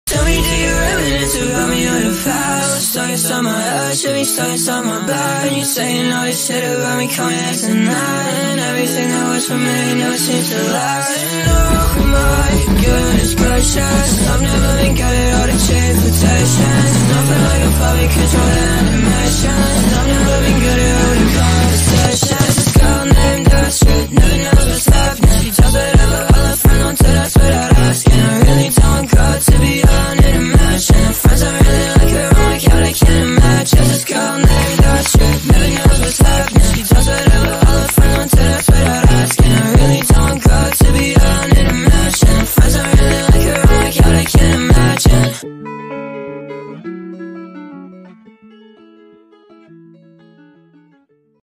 Astrid animation meme/amv
this song is terrible lets be honest with ourselves.

anyway, this is kind of a vent but in a funny way. I'm just a terrible texter. Someone will say something really sweet or emotional and I'll just be "k lmao"

sorry my upload schedule sucks, but I speedran all of this for you yesterday.